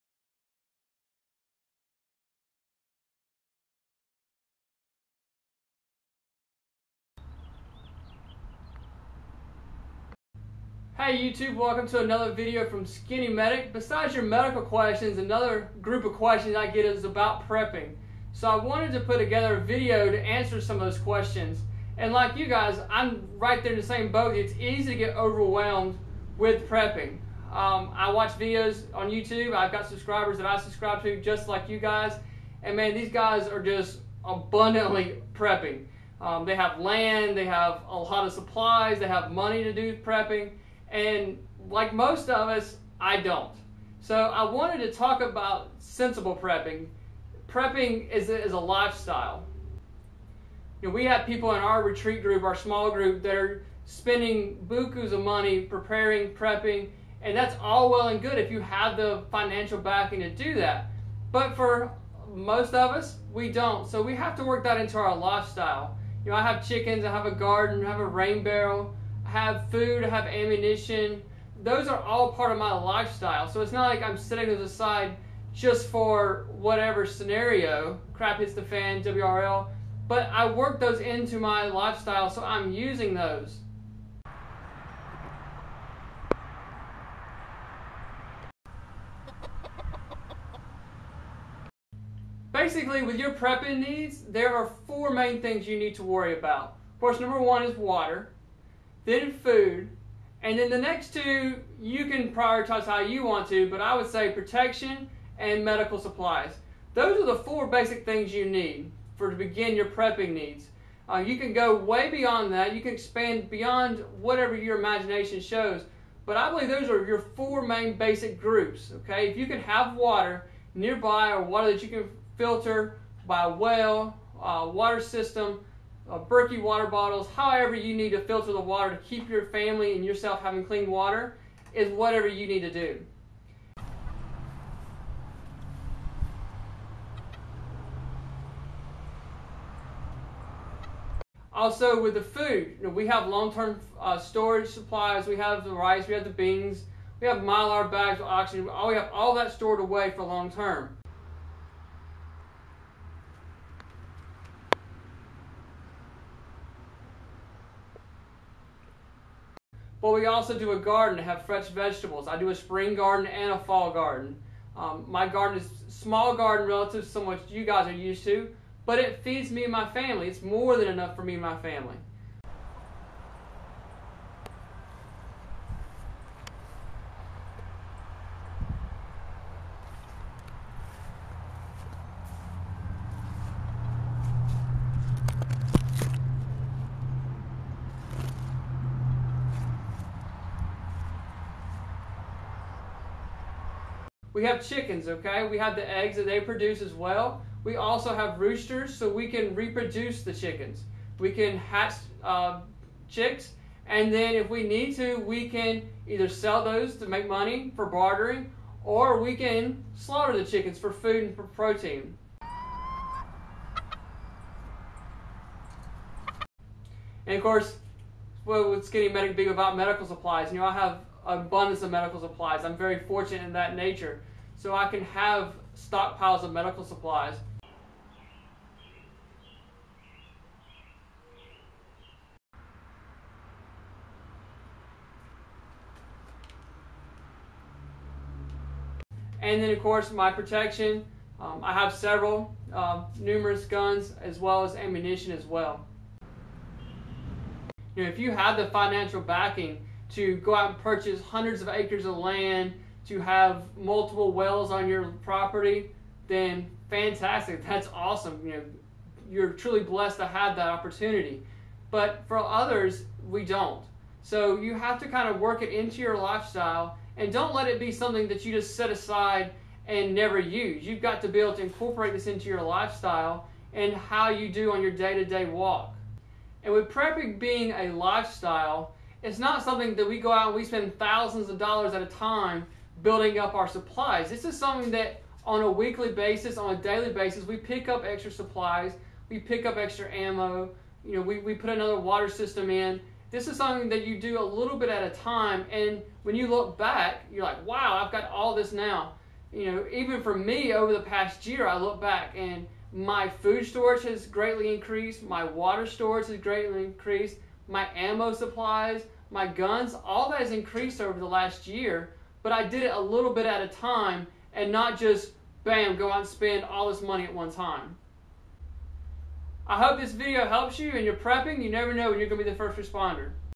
Sensible Prepping - Its a lifestyle!
You can go crazy with all your prepping! I believe in sensible prepping. Prepping being self reliant.